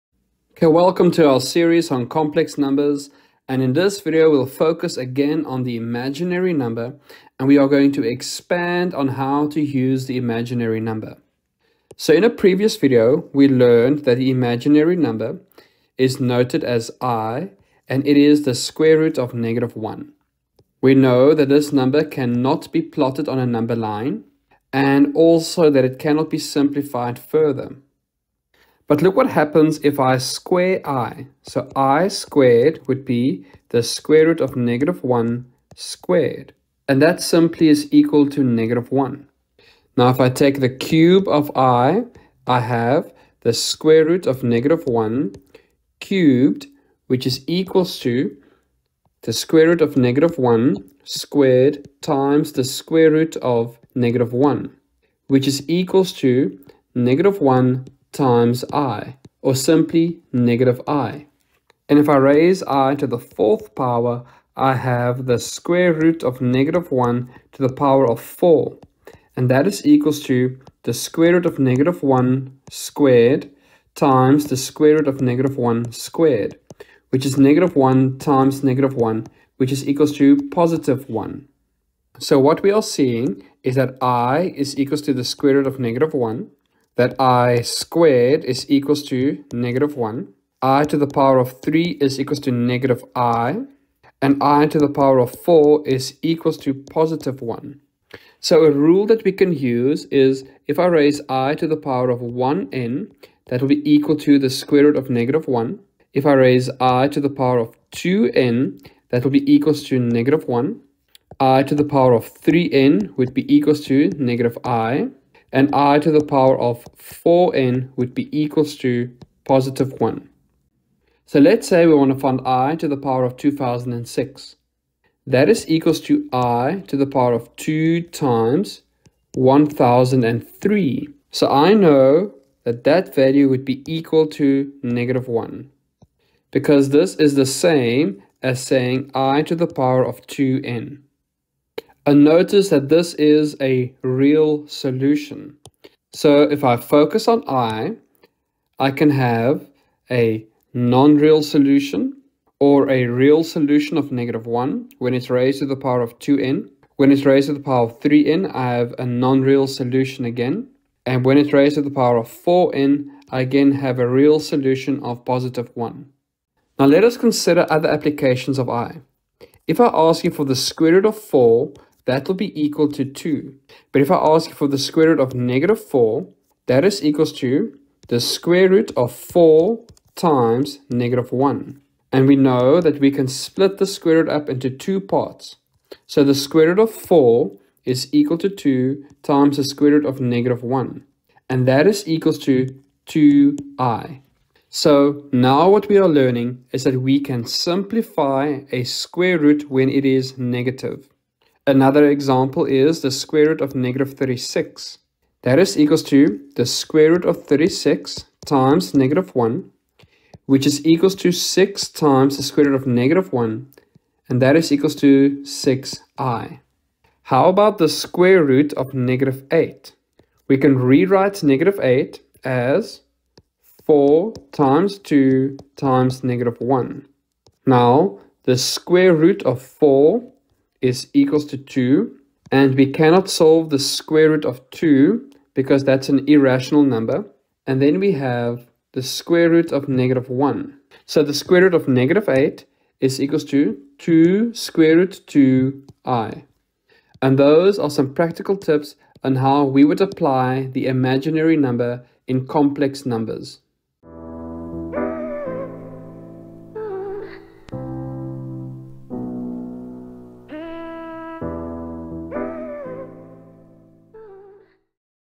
Advanced Math - Properties of imaginary numbers.
Welcome to Mathenatic! In this Math Tutorial we discuss how to use imaginary numbers in Complex numbers. Working with the imaginary number, i, is a fundamental concept to master before moving on to calculations with complex numbers.

This Mathematics Tutorial focuses on what happens when squaring i, and raising i to the power of 3 and to the power of 4. Raising i to various powers results in non-real solutions and real solutions.

This is for students studying Advanced Mathematics.